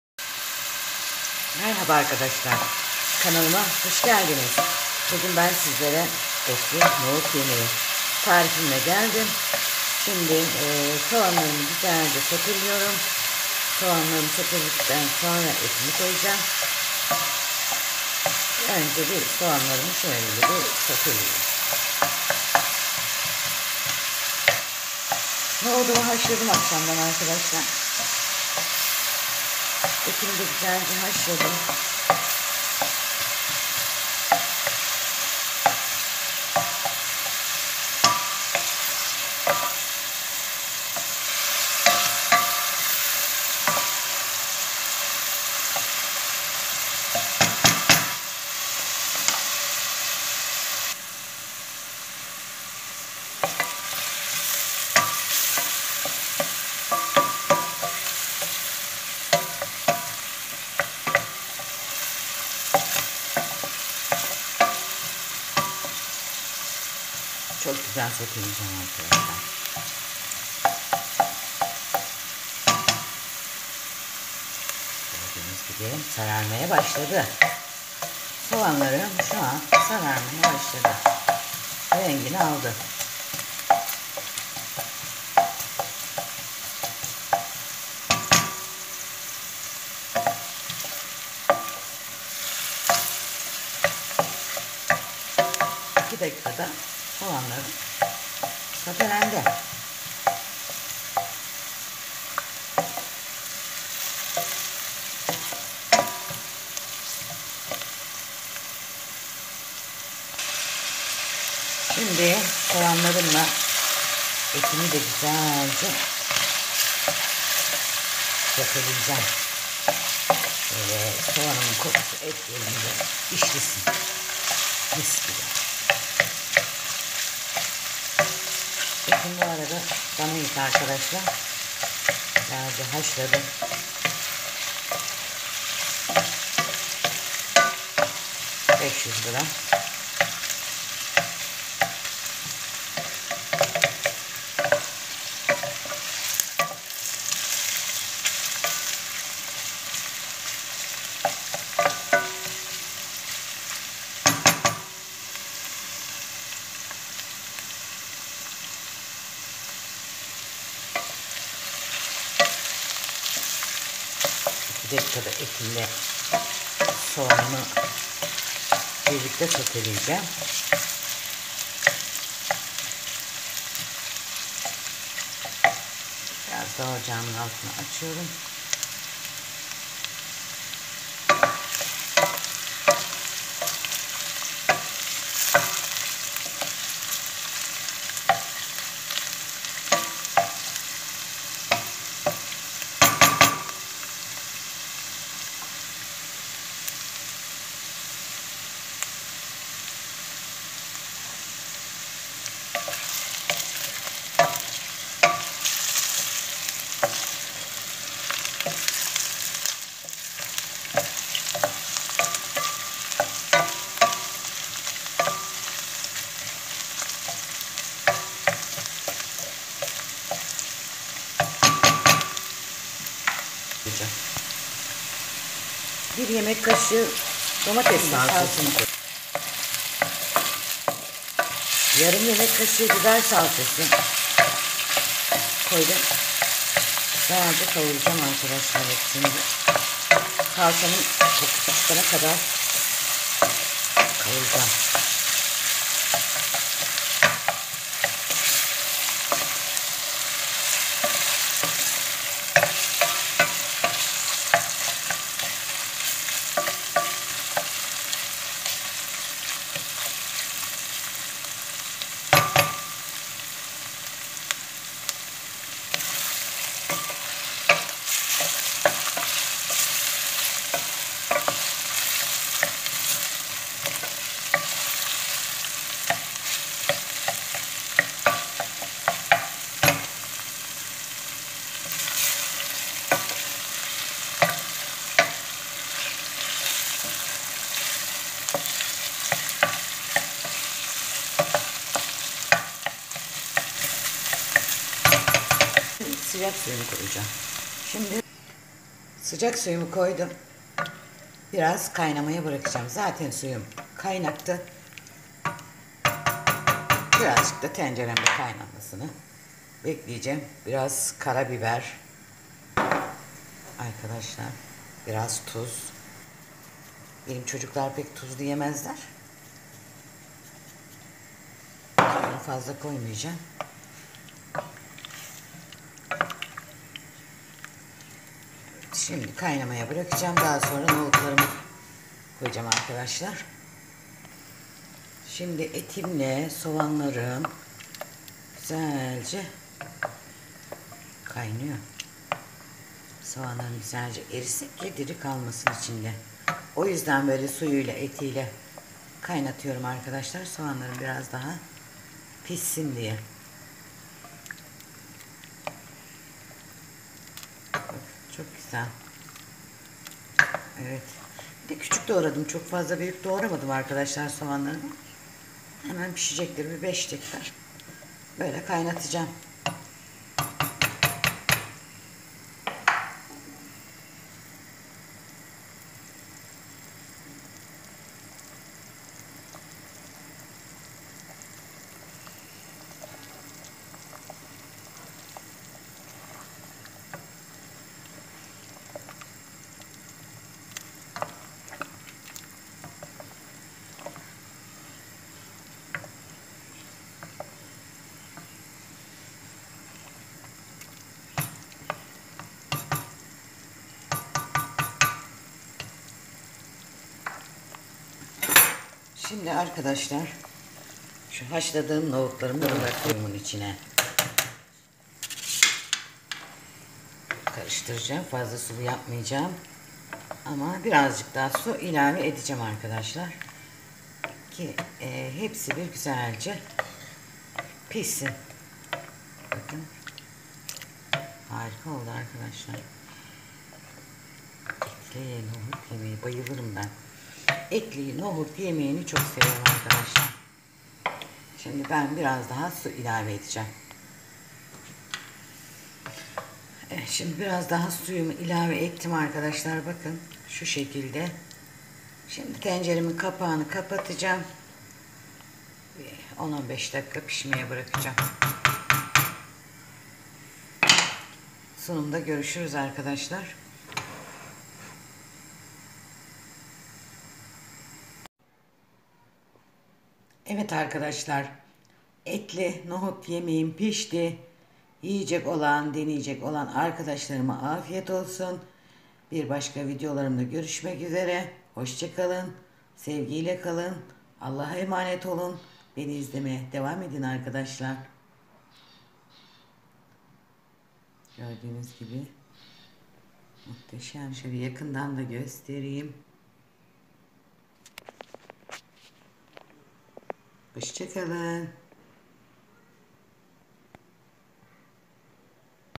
Kış aylarının vazgeçilmez nohut yemeği✅etli nohut yemeği tarifi😋
etlinohutyemeği etliyemektarifi etliyemekçesitler etsotetetarifi etlisebzeyemekleri etlikurufasülyeyemeğitarifi etliekmektarifi etlisuluyemekler etlievyemekleri etlilokantayemekleri nohutlubörektarifi.Malzemeler /2su bardağı nohut 500gr dana kuşbaşı et 1büyük boy soğan 1tatlı kaşığı karabiber 1yemek kaşığı salça yarım yemek kaşığı biber salçası yarım çay bardağı zeytin yağ ve ölçülü su